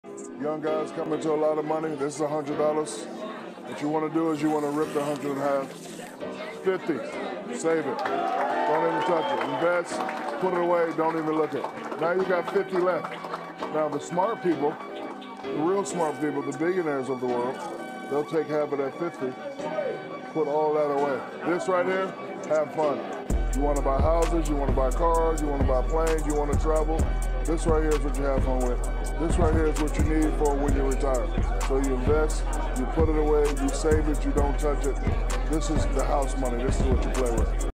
Shaq teaches you how to accumulate WEALH
Shaquille O’Neal shares the simple trick he used to become wealthy.